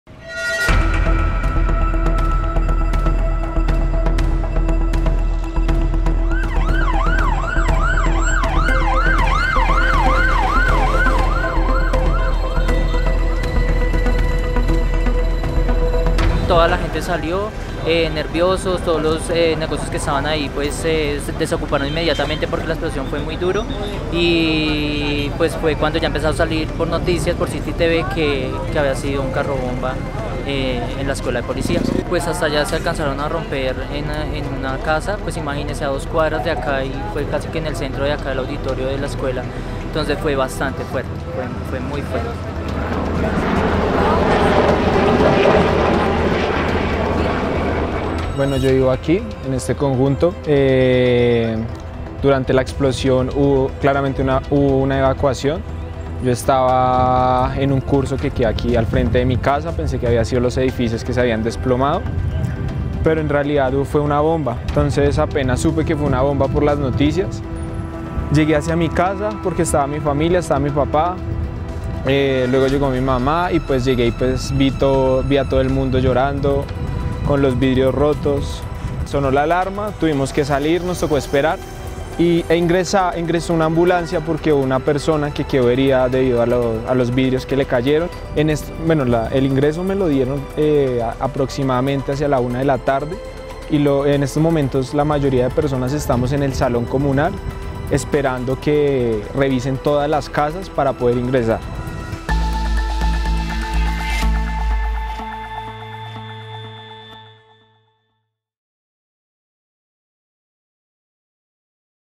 Así quedaron los barrios que están junto a la Escuela de Policía | EL TIEMPO
Más de tres horas por fuera de sus viviendas, con las mascotas en sus brazos, en pijama y anonadados. Así estaban los vecinos de los barrio Muzú y  Villa Mayor, que quedan junto a la Escuela de Cadetes de Policía General Santander.